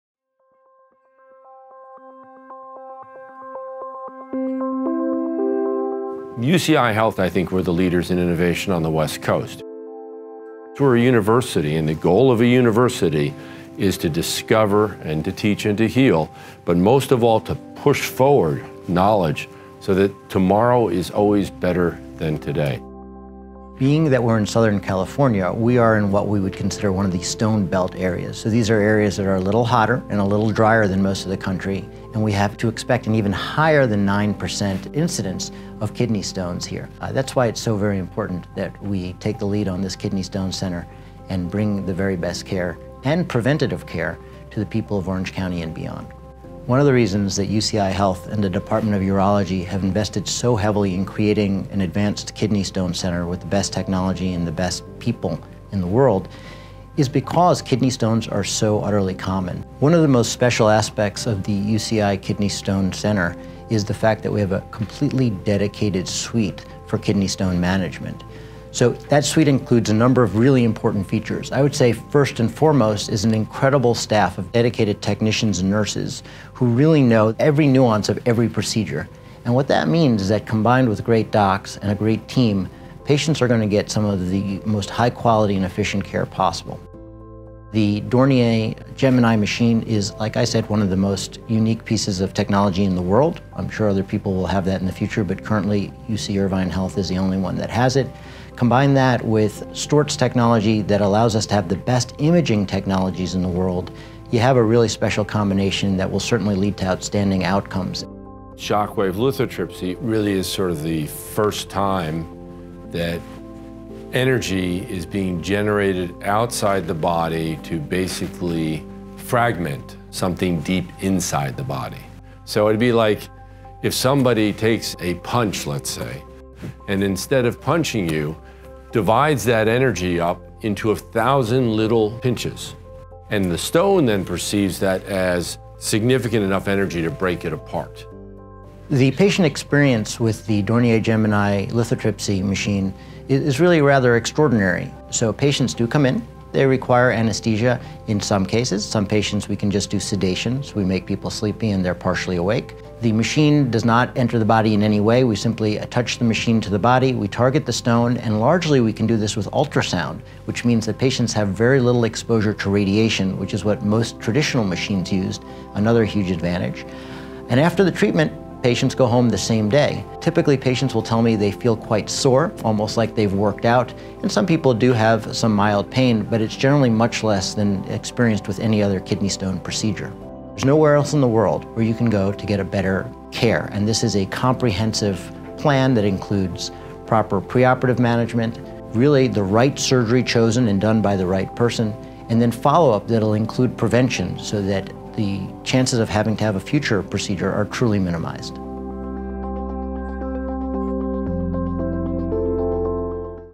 Treating kidney stones without surgery
Southern California is in the “stone belt region," which means we are especially susceptible to getting kidney stones. UCI Health has a highly skilled team of specially trained urologists who use the latest technology to break up kidney stones without surgery, using a sophisticated imaging and stone-fragmenting system called Gemini.

Get patient stories, wellness tips, health care news and more on our Live Well blog